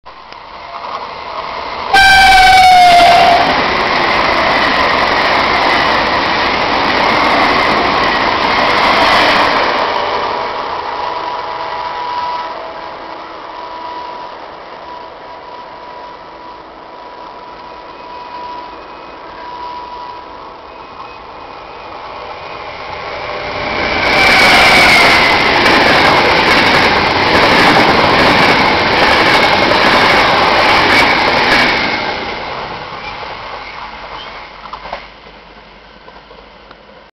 NTV Alstom AGV and a Trenitalia Commuter Train Near Florence, Italy (2)
Some time ago (I think maybe in 2014), my folks and I stayed briefly at Villa La Massa, located just outside of Florence, Italy (western outskirts of the city). Via della Massa, a small road which loops around the hotel's walls, provides a great spot for trainspotting--a nice mix of high speed and local commuter trains. Seating space is rather lacking (I had to resort to the painful guardrail), but the view is excellent!

Here is a nice meet of a Nuovo Trasporto Viaggiatori (NTV) Alstom AGV and a Trenitalia
FS Class E.402B.